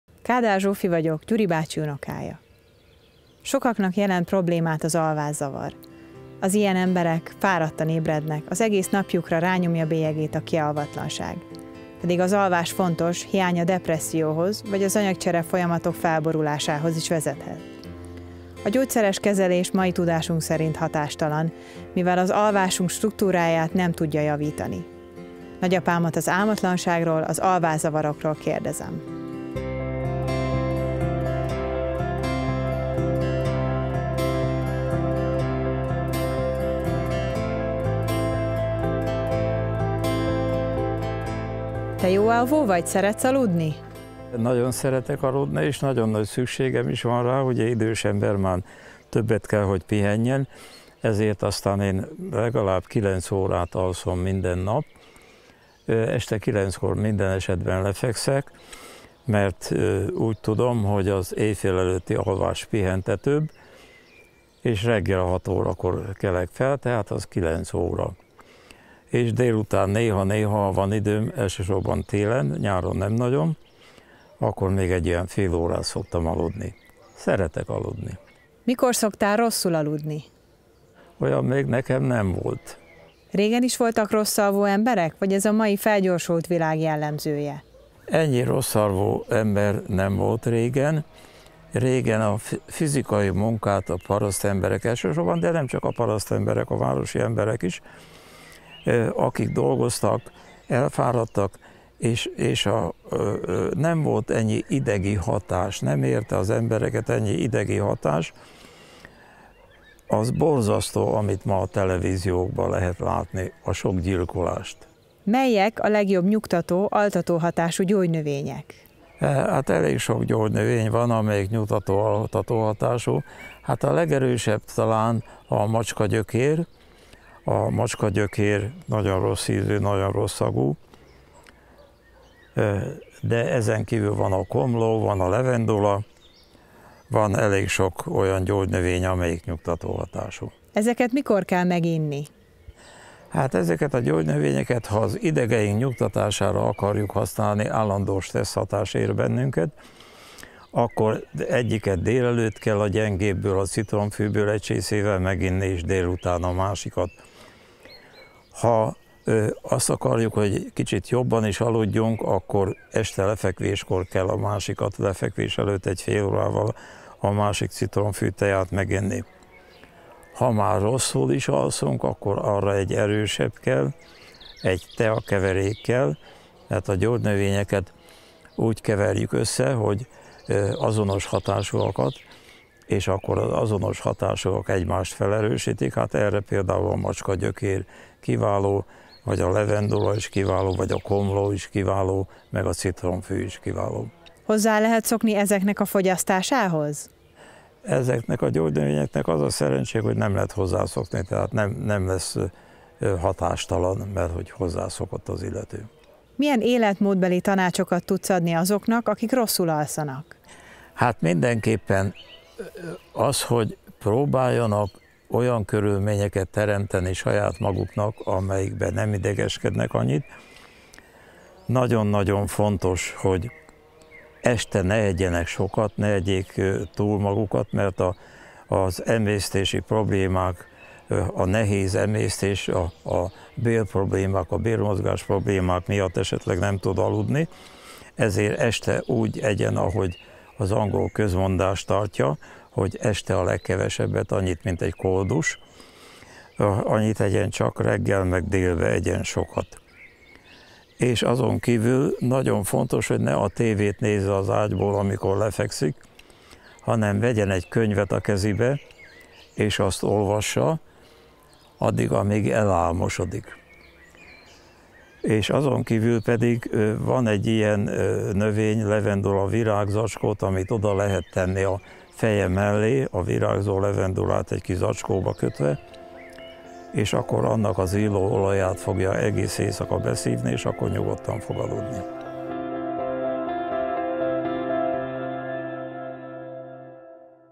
Álmatlanság, alvászavarok
Sokaknak jelent problémát az alvászavar. Az ilyen emberek fáradtan ébrednek, az egész napjukra rányomja bélyegét a kialvatlanság. Pedig az alvás fontos, hiánya depresszióhoz, vagy az anyagcsere folyamatok felborulásához is vezethet.